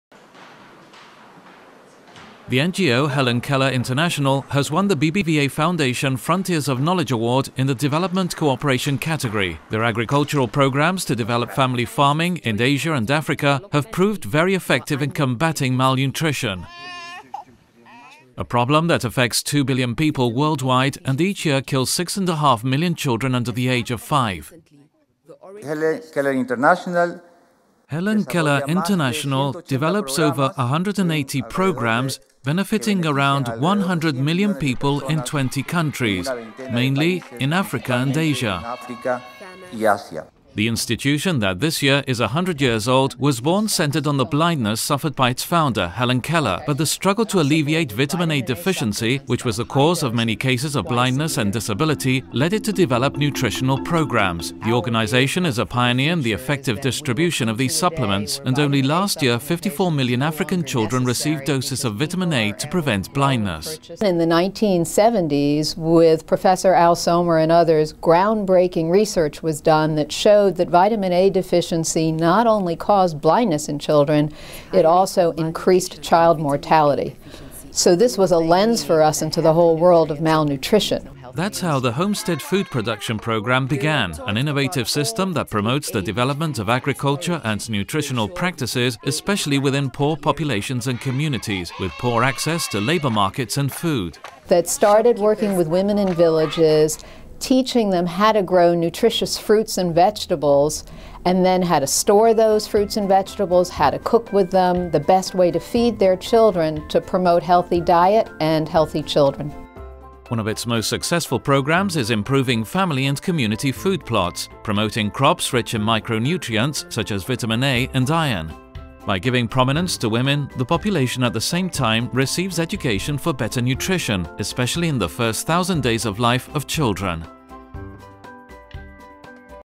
Helen Keller International, 2014 BBVA Foundation Frontiers of Knowledge in Development Cooperation
The BBVA Foundation Frontiers of Knowledge Award in the Development Cooperation category goes in this seventh edition to American NGO Helen Keller International (HKI) for agricultural programs that help families and villages to raise their own nutritious foods. “Hunger and low dietary diversity reduce cognitive function, physical capacity, resistance to disease and quality of life and lifetime earnings. Heller Keller International champions Homestead Food Production, an innovative, interdisciplinary program that promotes improved agricultural and nutritional practices in a synergistic fashion. This approach is mostly applied to communities that have difficult access to labor and food markets,” in the words of the jury’s citation.